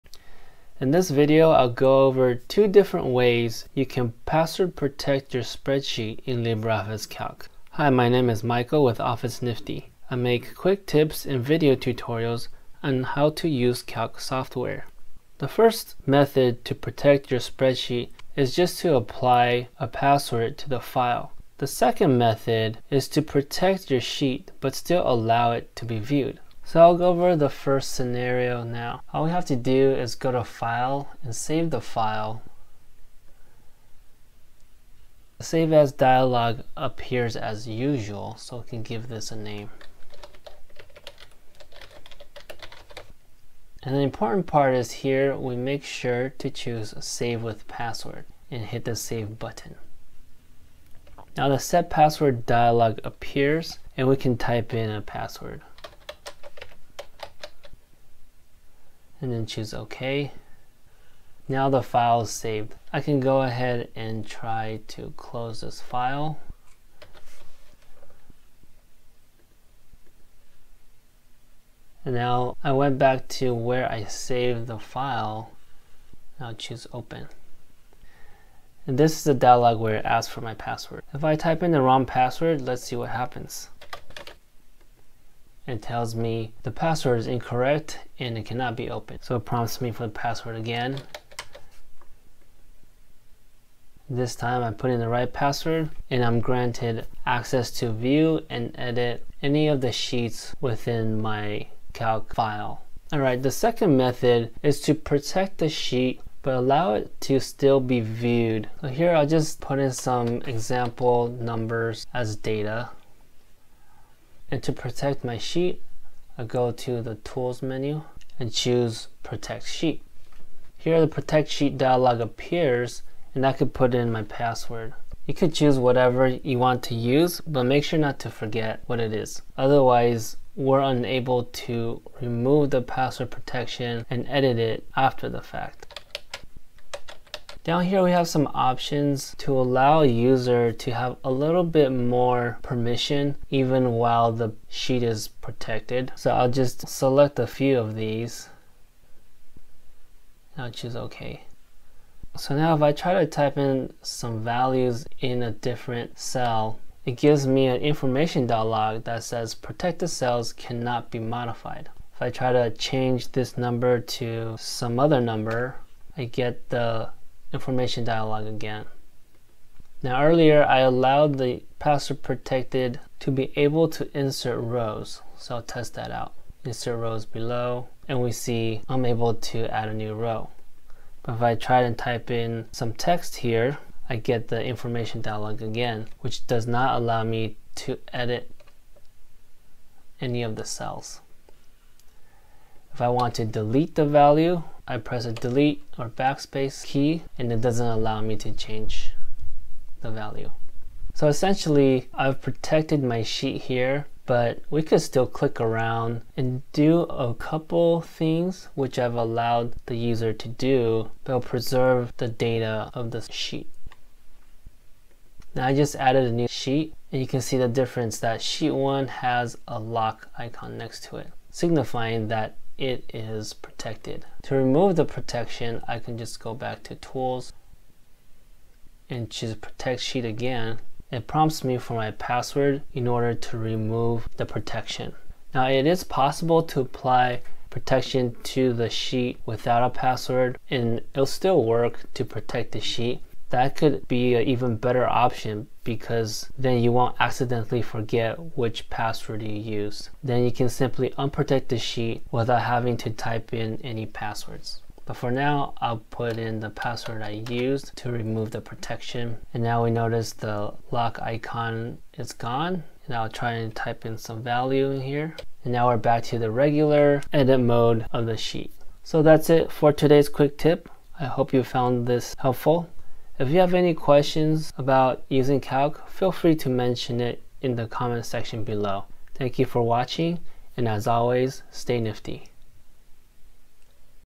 How to Password Protect Your Spreadsheet | LibreOffice Calc
This video shows how to add password protection to your spreadsheet. It's a way to protect your sheet.

Permissions can also be set to allow users to do these specific things:
- Select protected cells
- Select unprotected cells
- Insert columns
- Insert rows
- Delete columns with unprotected cells
- Delete rows with unprotected cells
- Use AutoFilter
- Use Pivot Table and Pivot Chart

Timestamps
0:00 Method 1- Save with password checkbox
1:43 Method 2- Protect Sheet dialog
3:44 Remove the protection